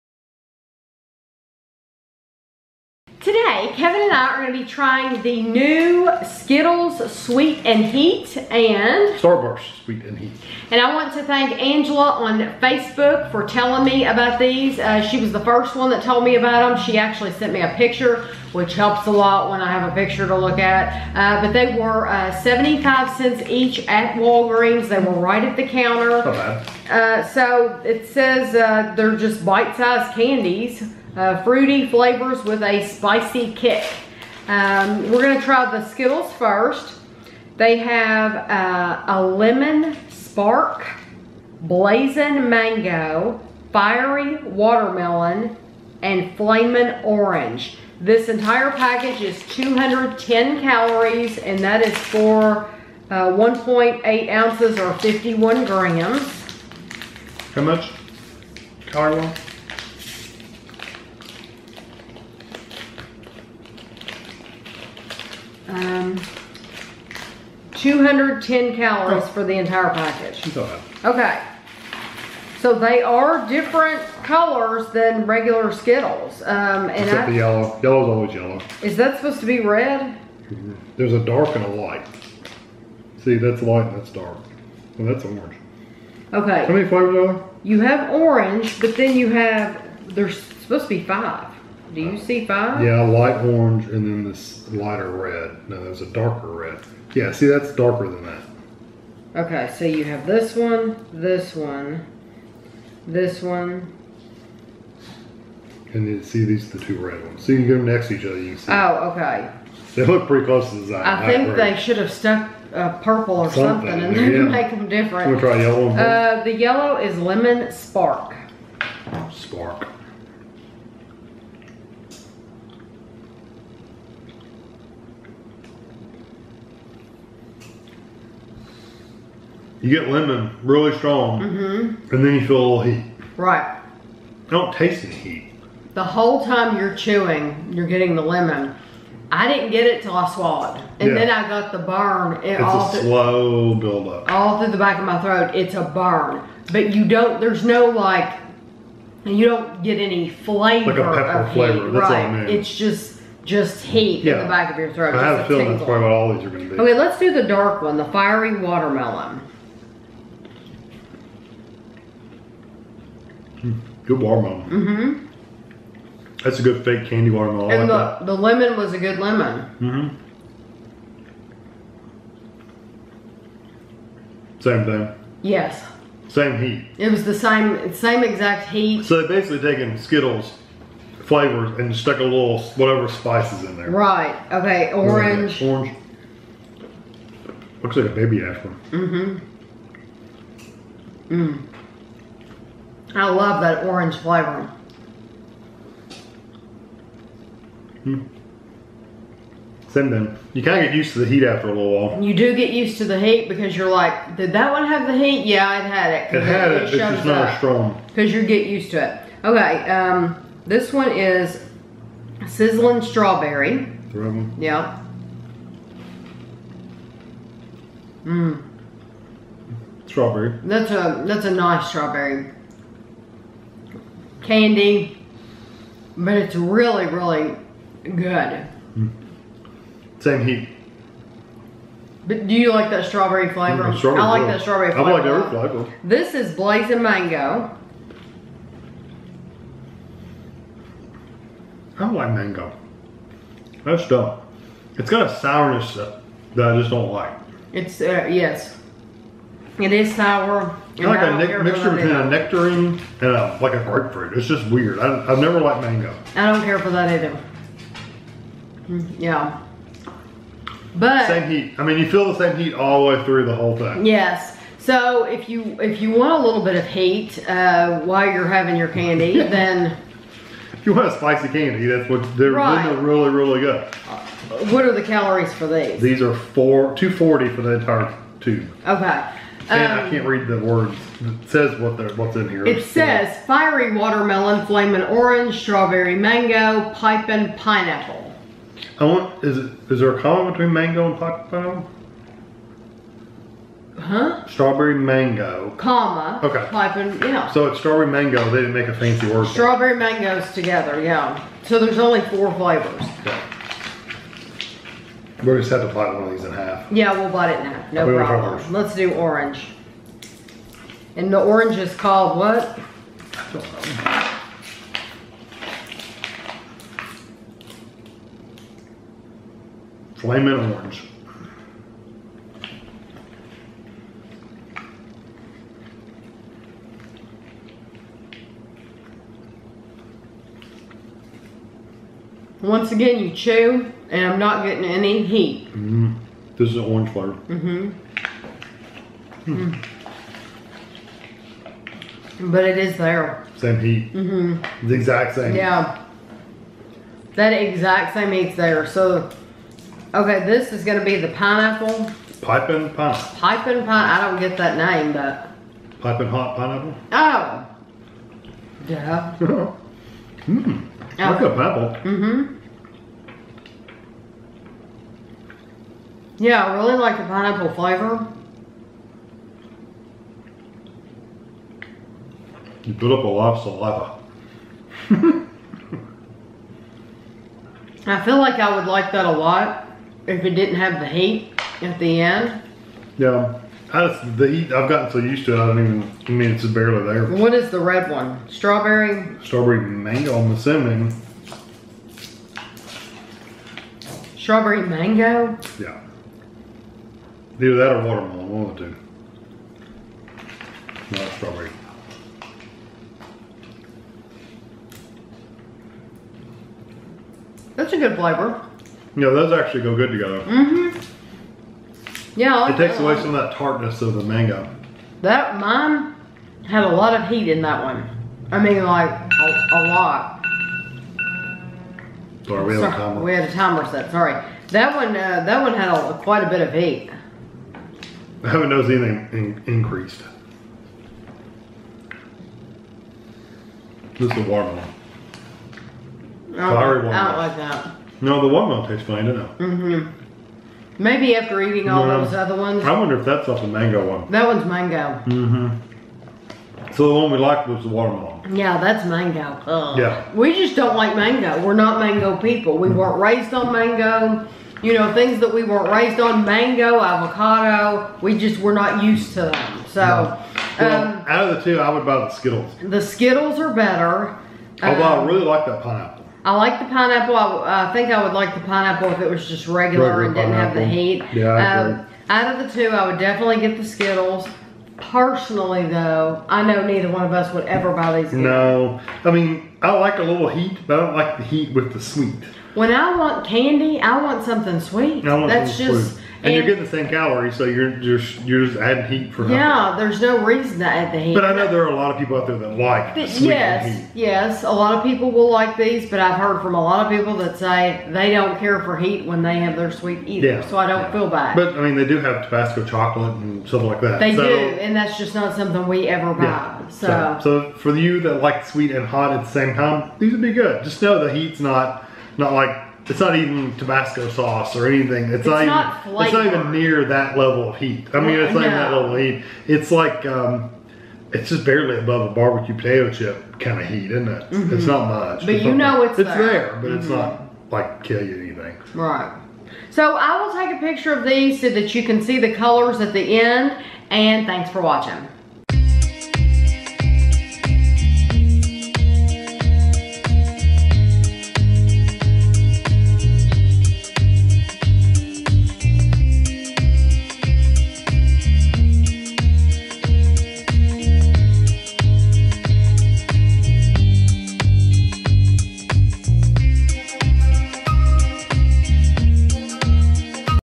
Sweet Heat Skittles & Sweet Heat Starburst Review
This is a taste test/review of the new Sweet Heat Skittles and Sweet Heat Starburst. They were 75 cents each at Walgreens.

* Skittles Flavors: Lemon Spark, Blazin’ Mango, Fiery Watermelon and Flamin’ Orange. 1.8 oz. (51g) = 210 calories
* Starburst Flavors: Fiery Watermelon, Flamin Orange, Strawberry Mango and Pipin’ Pineapple. 1 package (58.7g) = 240 calories